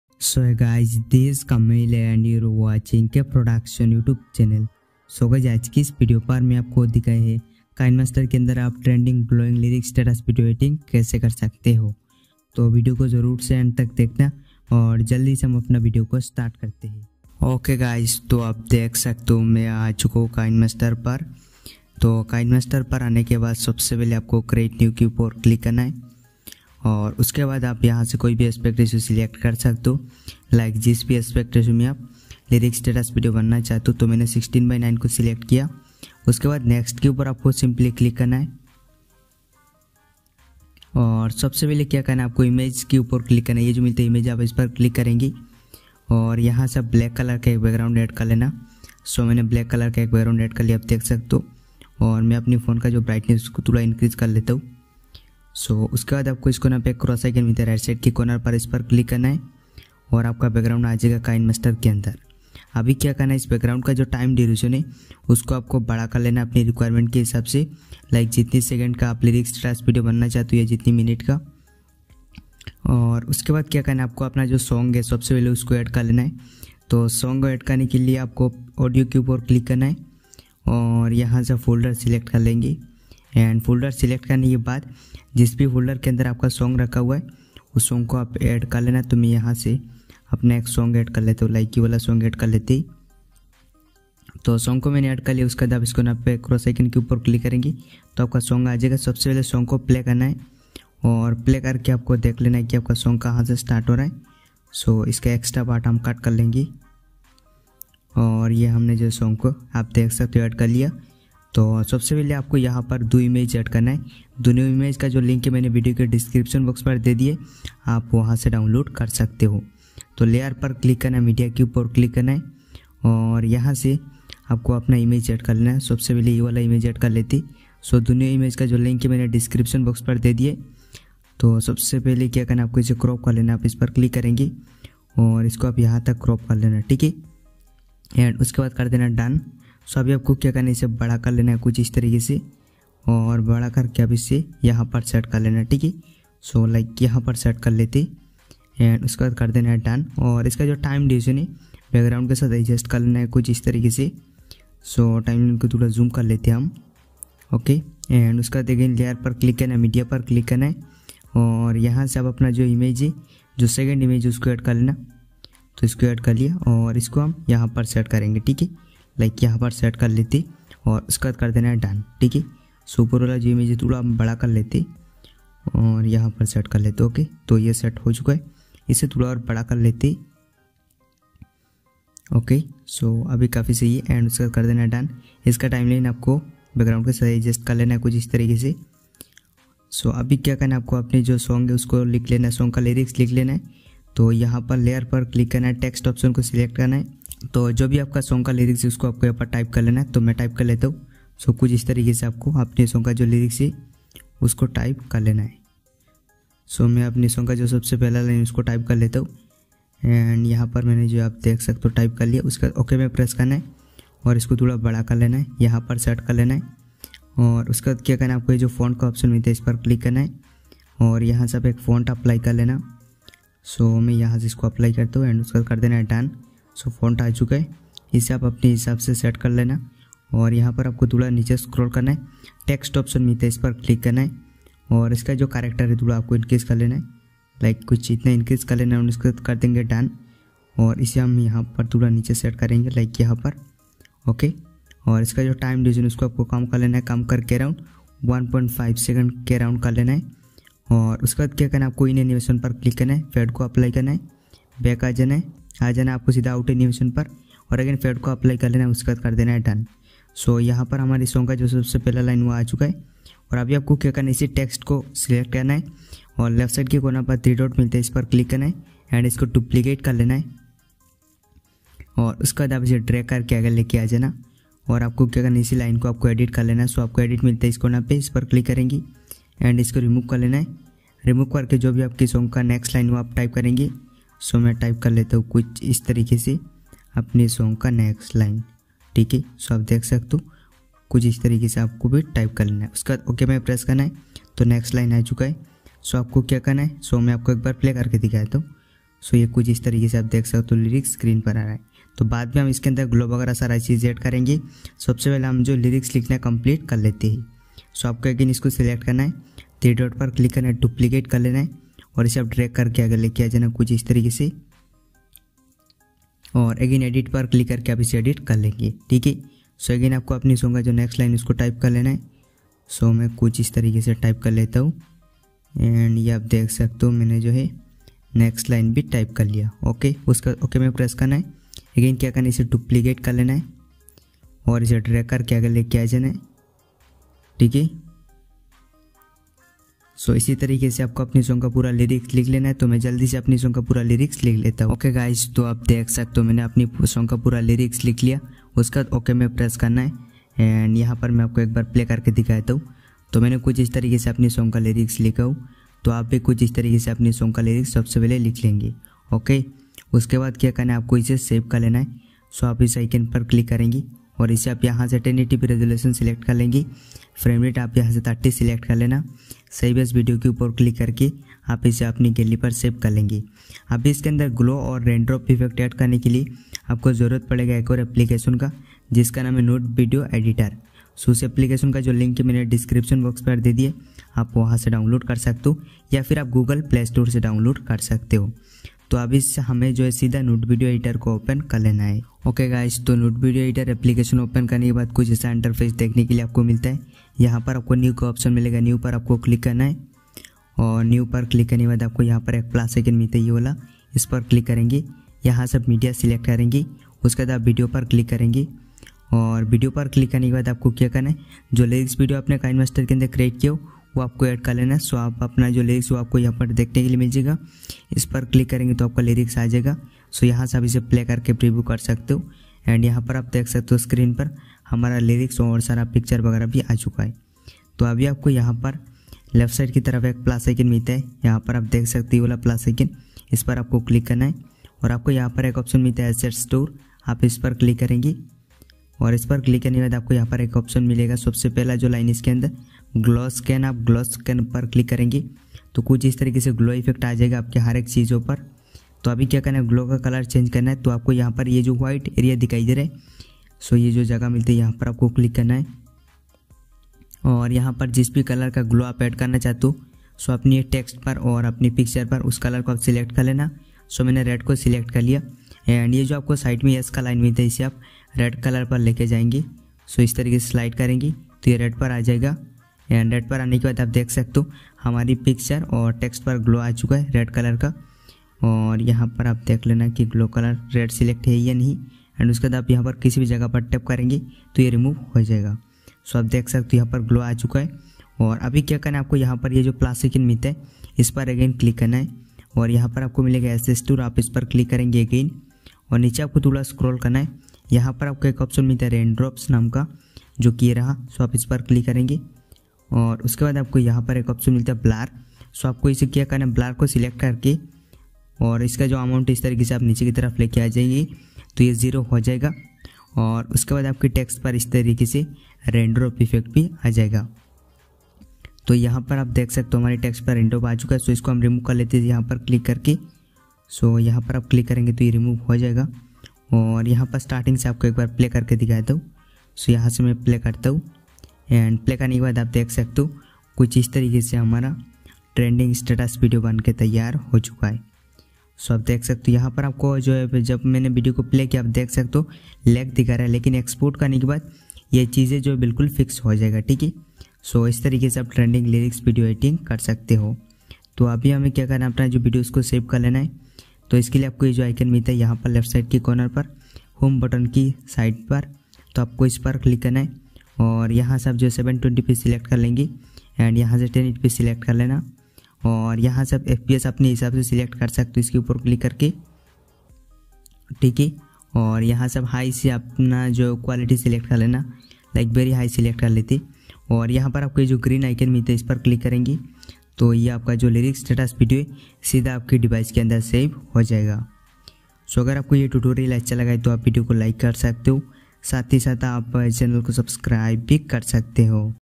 Trending Glowing Lyrics Status Editing In Kinemaster | Rain Lyrics Status Editing In Kinemaster
Hello friends welcome to our channel ka production doston aaj ke iss video par main aapko dekhiya hai kinemaster me trending glowing rain drop lyrics status editing kaise kar sakte ho...